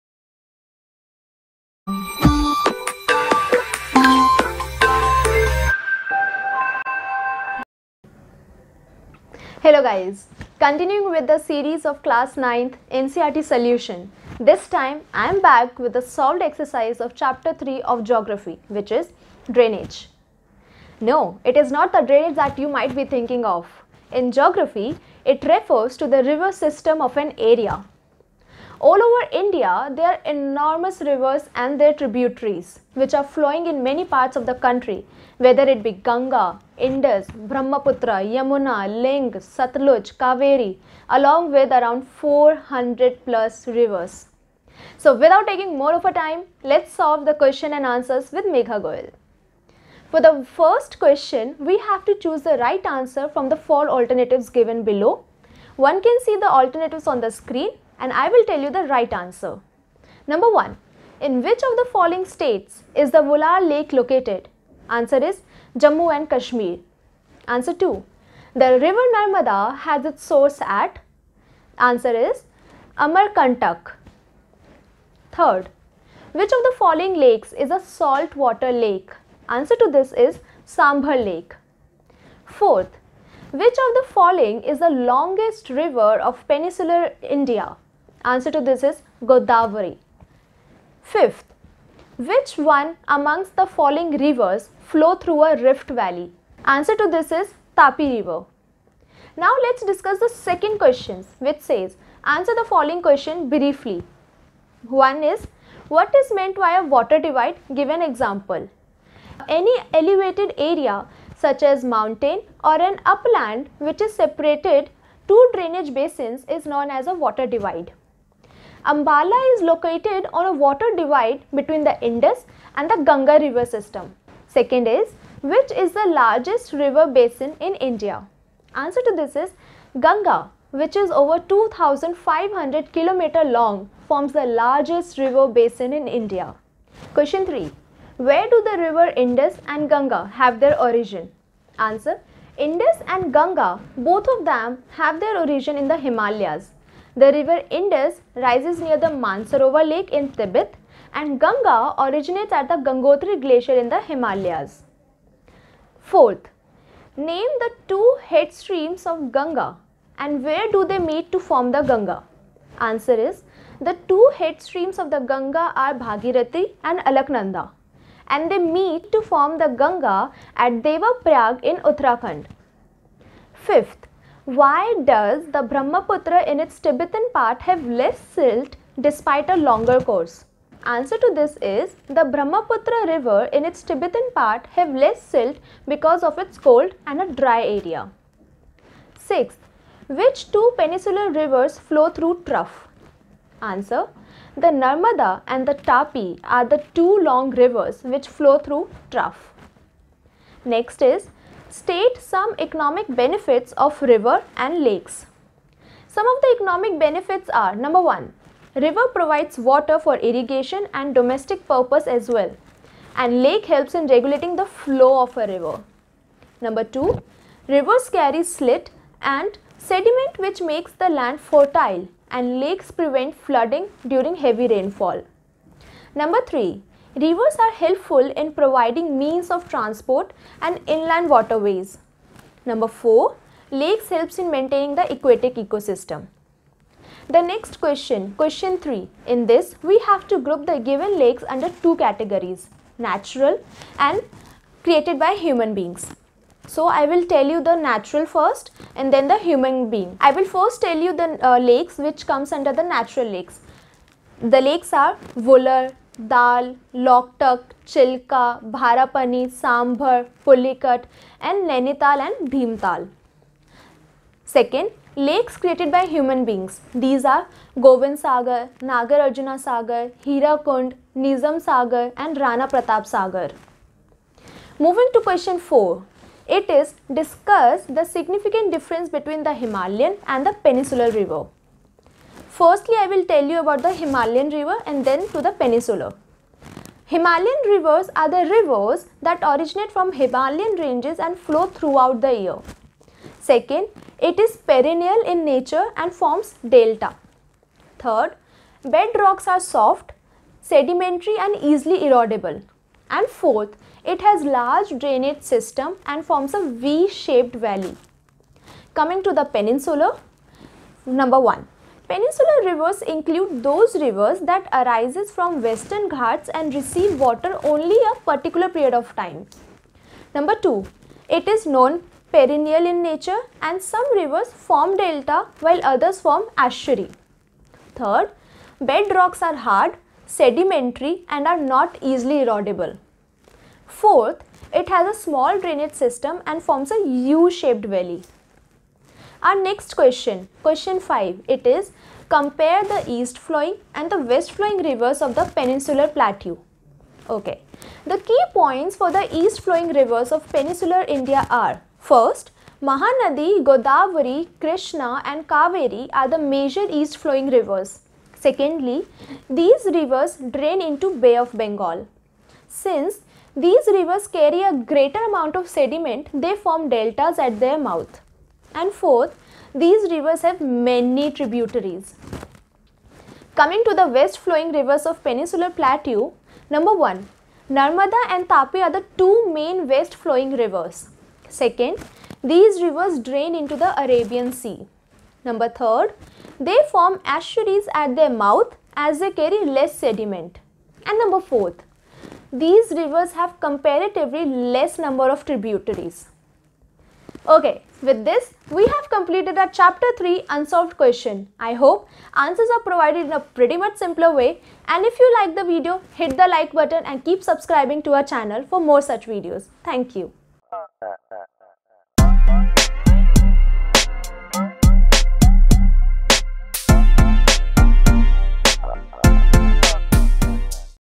NCERT Solutions Class 9 Social Science (Geography) Chapter 3 Drainage
Class 9th NCERT Social Science( Contemporary India ) Chapter 3 Drainage

This video provides the solutions of chapter 3 Drainage from textbook ‘Contemporary India (Geography) ’. It includes answers to all the questions of the chapter given in the back exercise.

The video starts with a short summary of the chapter, which will help students to understand the lesson then followed by solutions to textbook questions to help students relate and understand solutions easily. Solutions to exercises including Short Answer and Long Answers have been covered with detailed explanation.